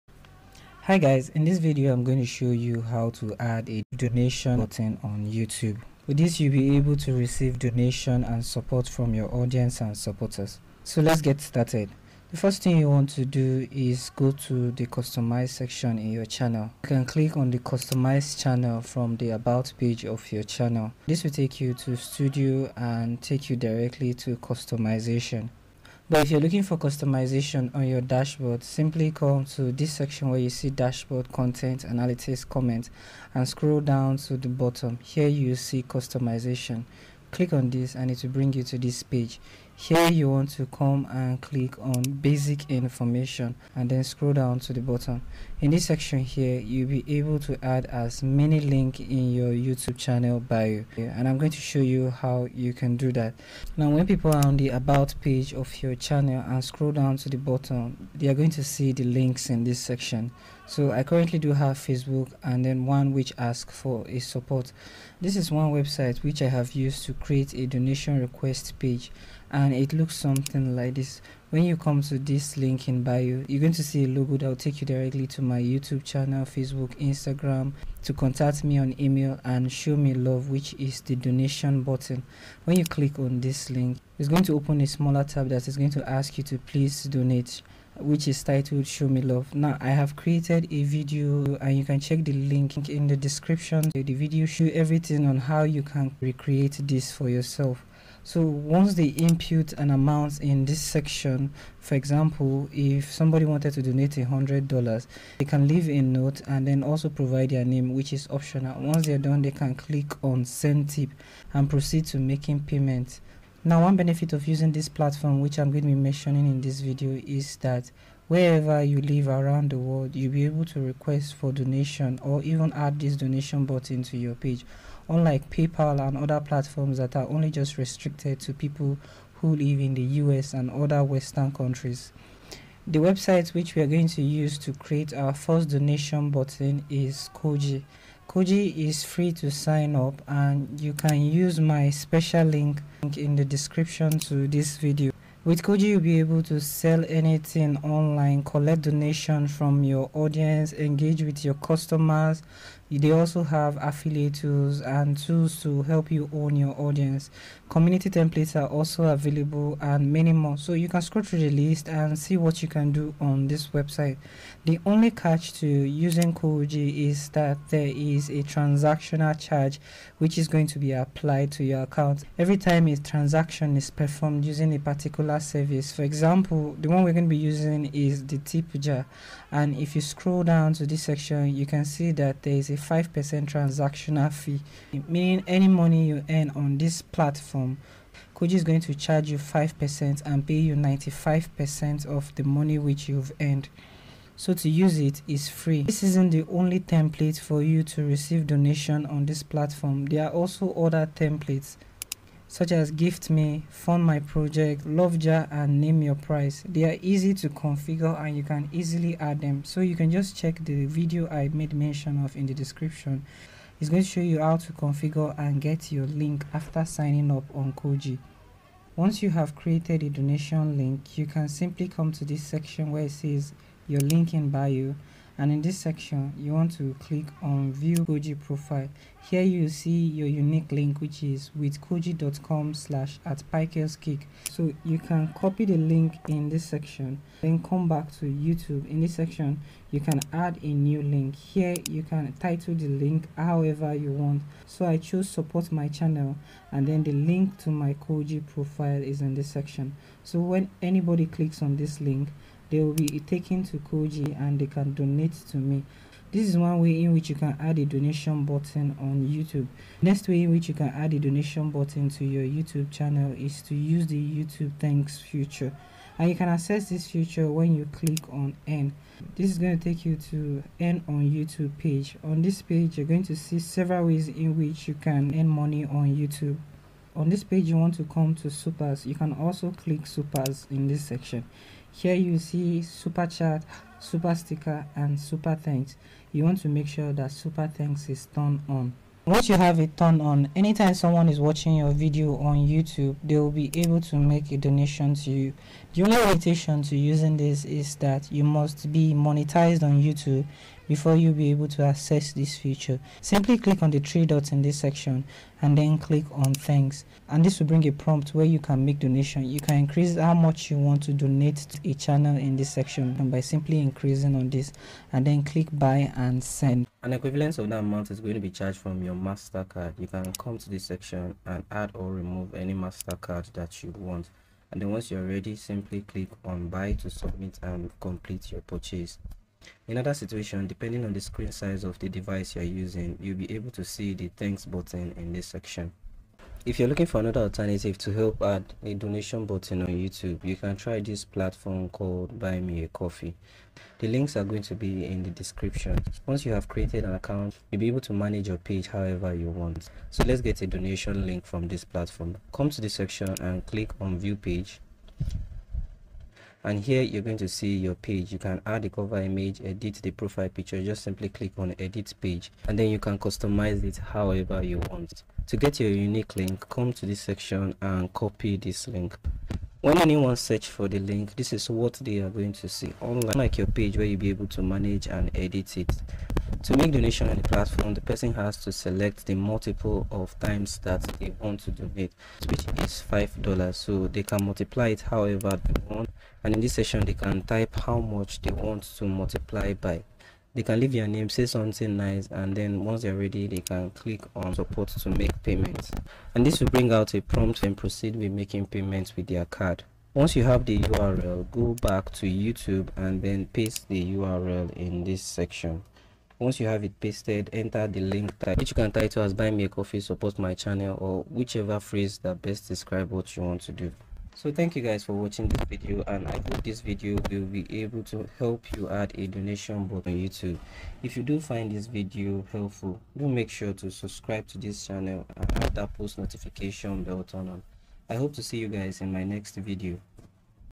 How to Add Donation Buttons on YouTube - Full Guide
Looking to support your cause through your YouTube channel? In this video, "How to Add Donation Buttons on YouTube - Full Guide," we will show you step-by-step how to add donation buttons to your YouTube videos and channel. Learn how to enable this feature to allow your viewers to contribute directly to your cause, whether it’s for charity, personal projects, or any other fundraising purpose. This comprehensive guide will help you maximize your fundraising potential on YouTube.

Fundraising with YouTube